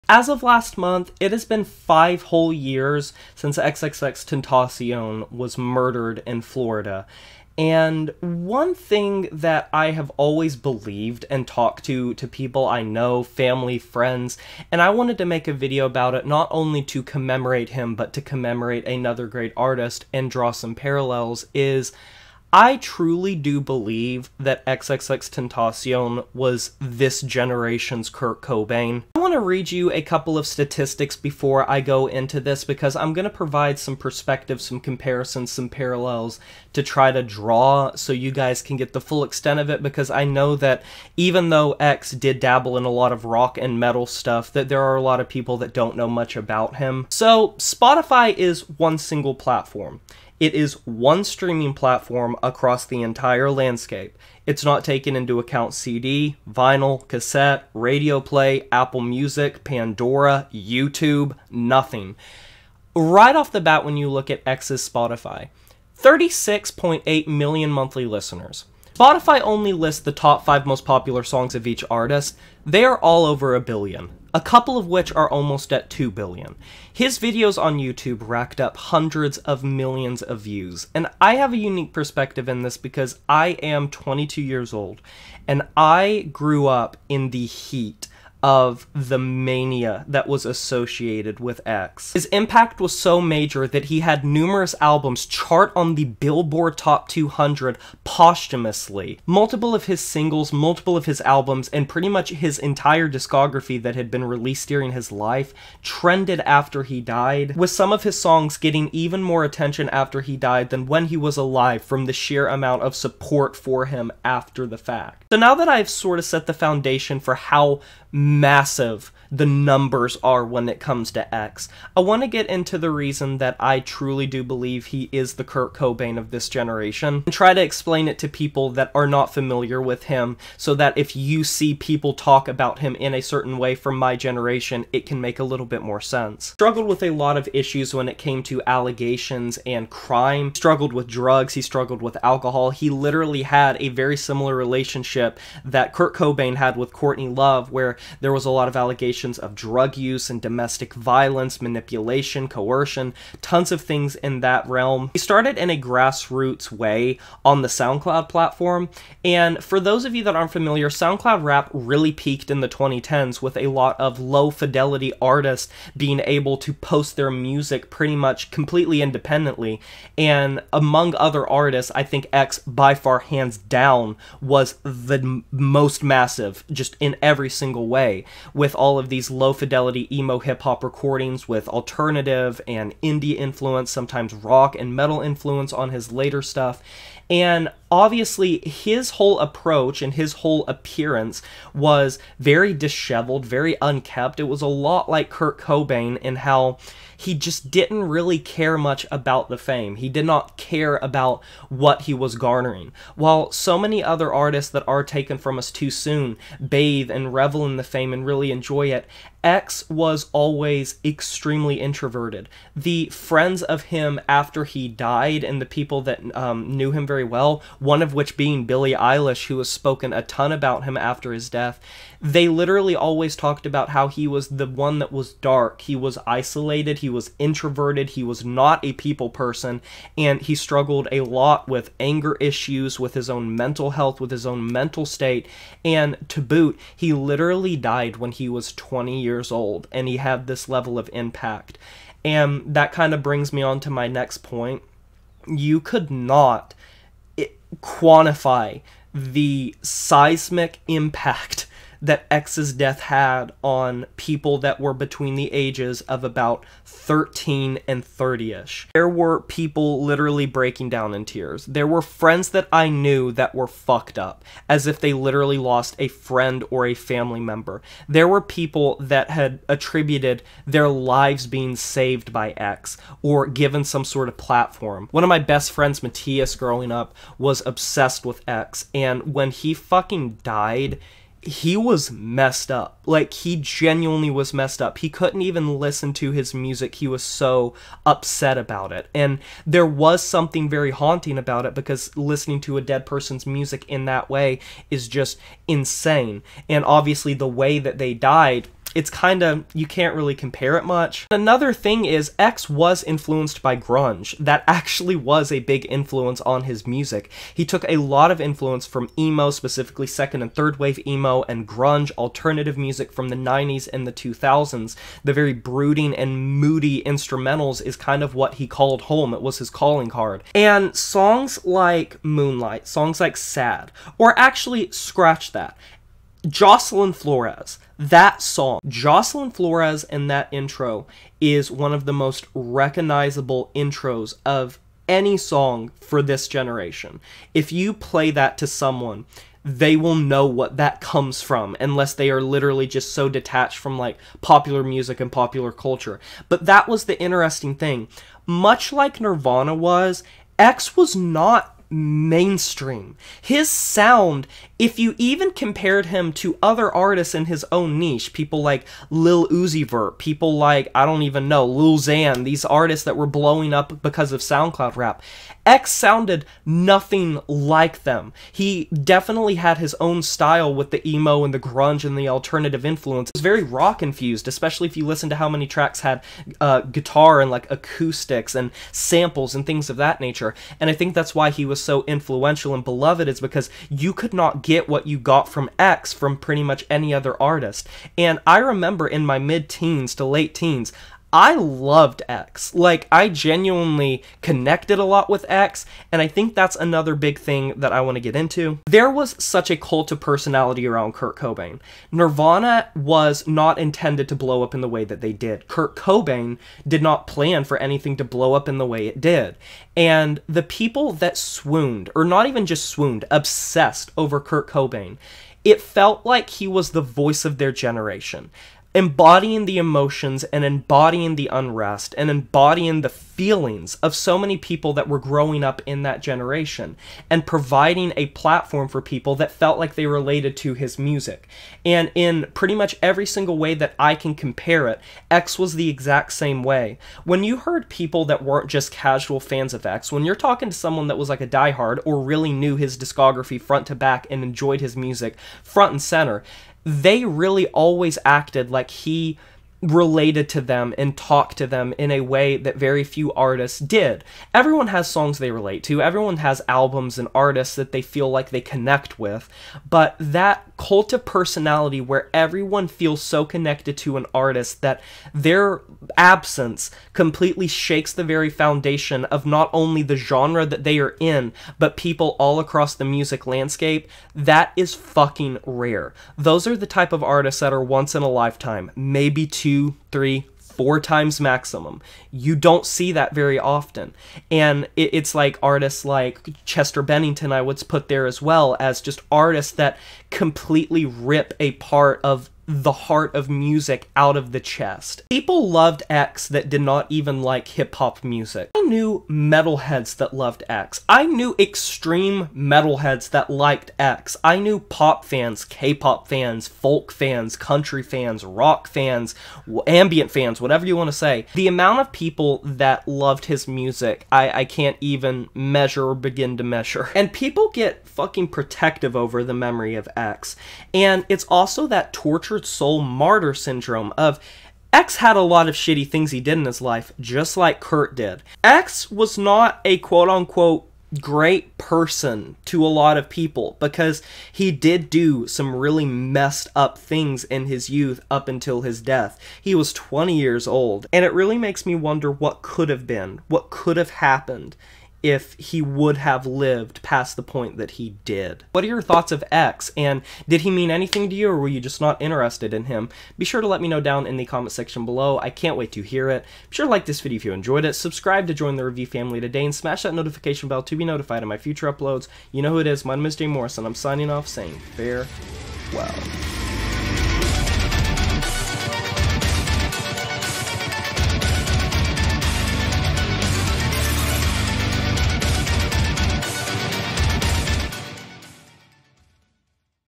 Why XXXTentacion Is This Generation's Kurt Cobain
XXXTentacion was the voice of the current generation, relating to them in such a profound way and leaving a noticeable rift in the modern musical landscape in his absence. Kurt Cobain was the troubled soul that never wished for the fame, but was handed the platform of his entire generation's youth. The tragic lives of XXXTentacion and Kurt Cobain parallel each other in such a strong way. In this video, I go over why that is.